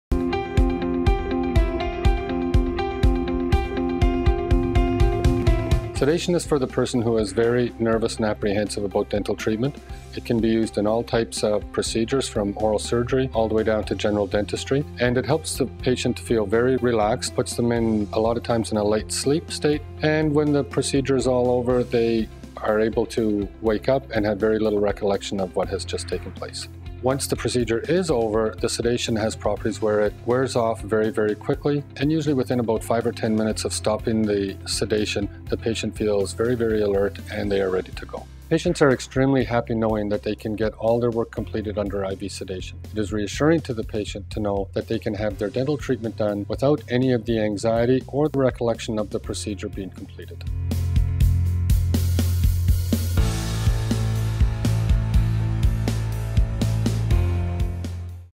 Sedation Dentistry 1:00 Sample
Sedation Dentistry. What is it? How does it work? Here's a sample.